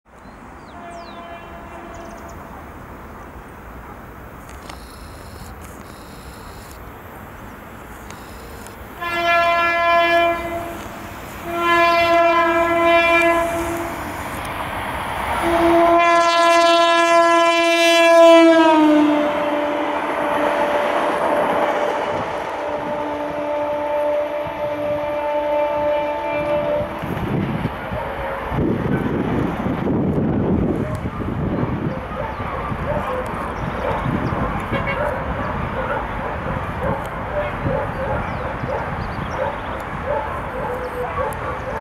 [FULL HD] Trenes Argentinos (LM) - ALCo DL535T D659 con el pata de perro por Pacheco!!
Nos encontramos ubicado en la estación Pacheco y podemos ver a la pequeña ALCo DL535T D659 con el servicio Villa Ballester - Zarate (Apodado como el Pata de Perro) con destino a Villa Ballester.